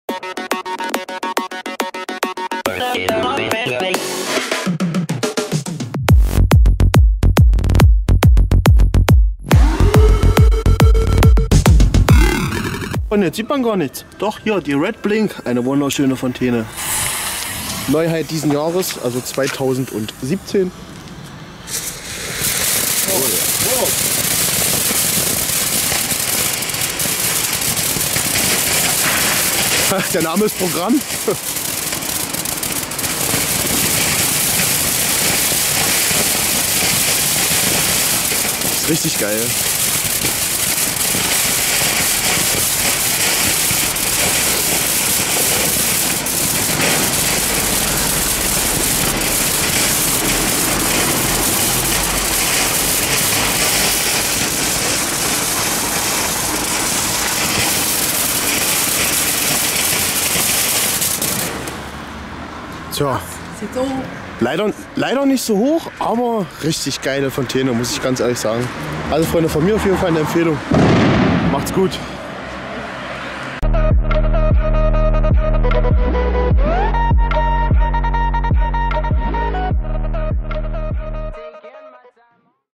Empfehlungsalarm Lesli Red Blink Fontäne
Monster Mäßig geil das teil Oder?
Preis 4-5€
Gewicht: 400 g
NEM: 85 g
Hersteller: Lesli Feuerwerk
Gefahrgutklasse: 1.4G
Kategorie:  2
Brenndauer:  45 s
Effekthöhe:  2 m
Schusszahl:  1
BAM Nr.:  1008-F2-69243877
Abmessungen:  60x155 mm